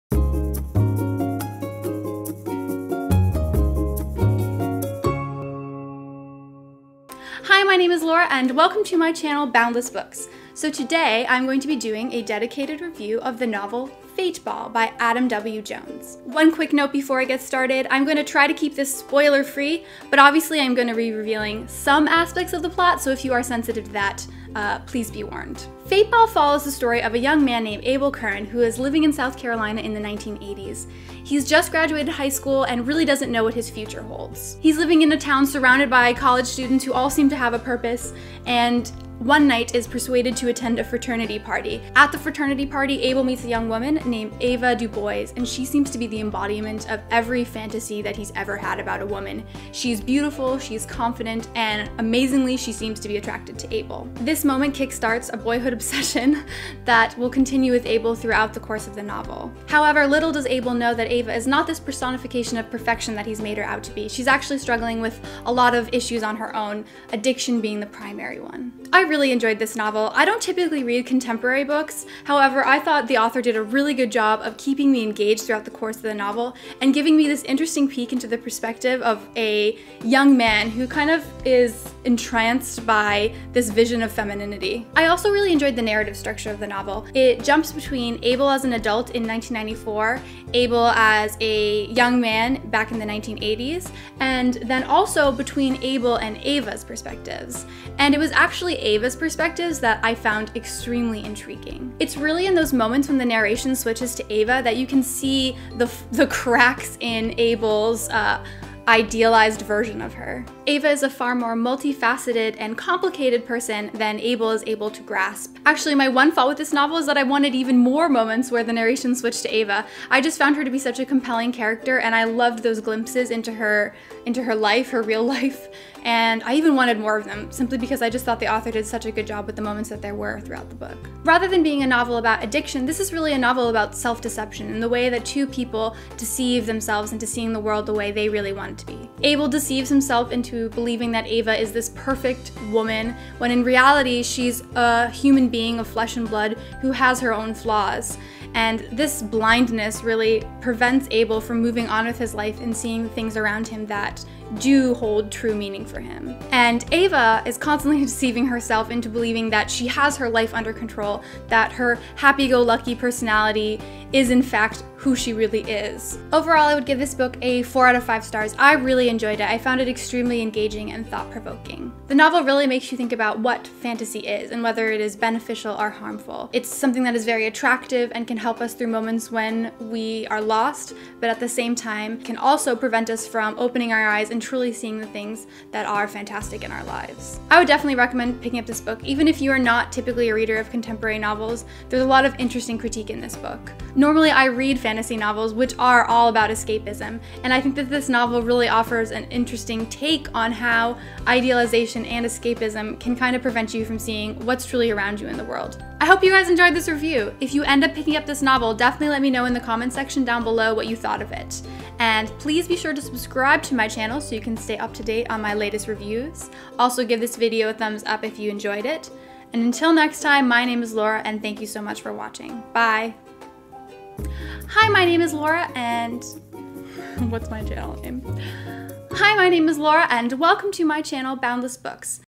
Book Review | Fate Ball by Adam W. Jones
Wisdom House Books was kind enough to send me the novel Fate Ball by Adam W. Jones for review. This contemporary novel follows a young man name Able Curran and his idolization of the enticing but very troubled Ava Dubose.

And if you want to stay up-to-date with all of the books that I'm currently reading, follow me on Goodreads!